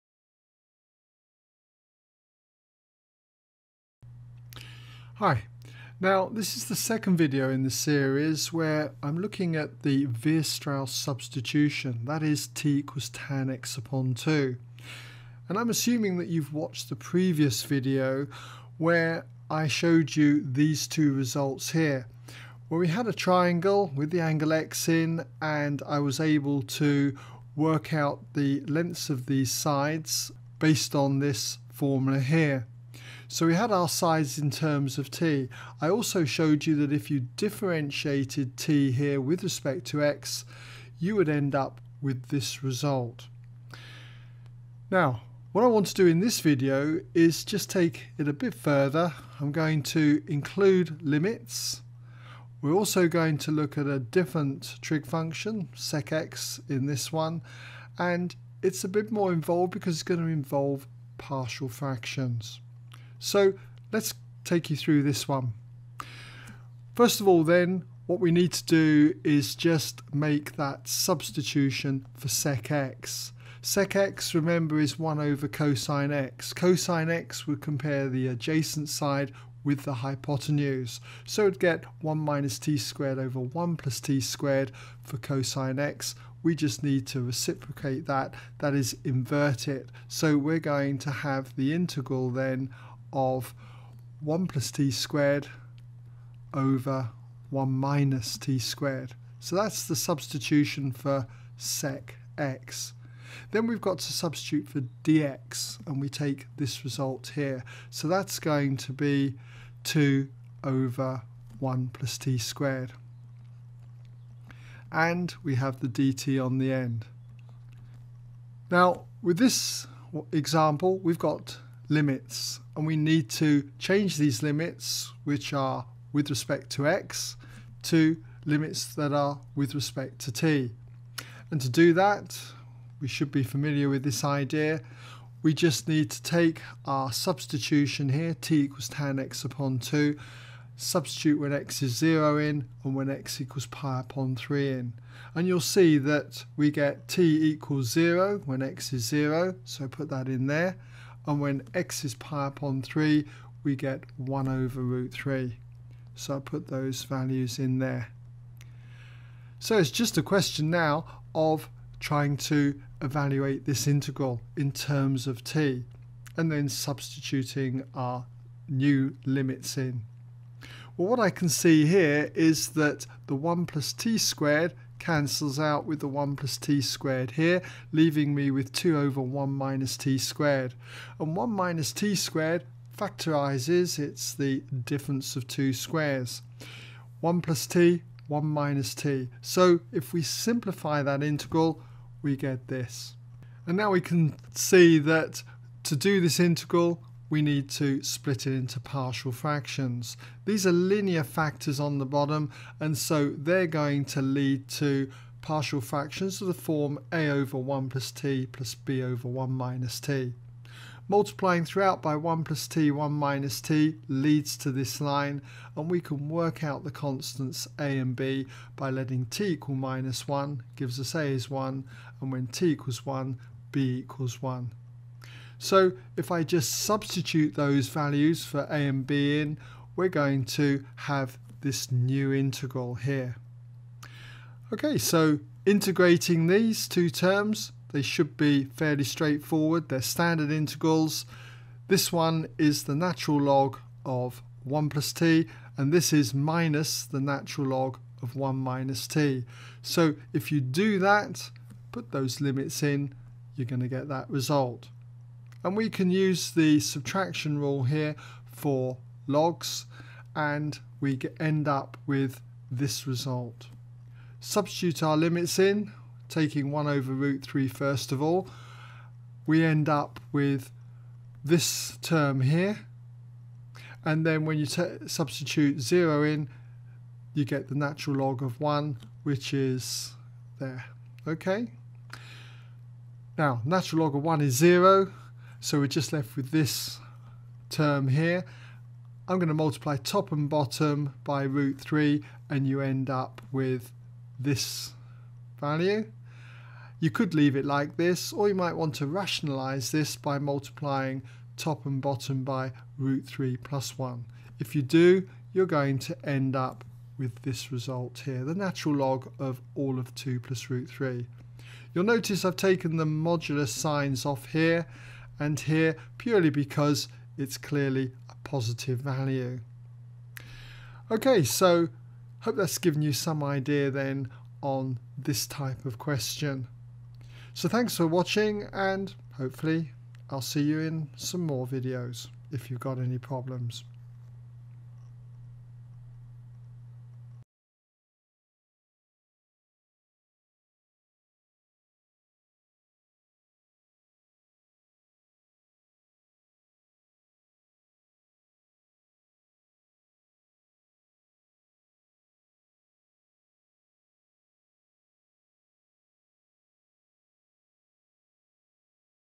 The Weierstrass Substitution (part 2) | ExamSolutions
This is the second video where you are shown the Weierstrass Substitution to help solve trigonometric integrals.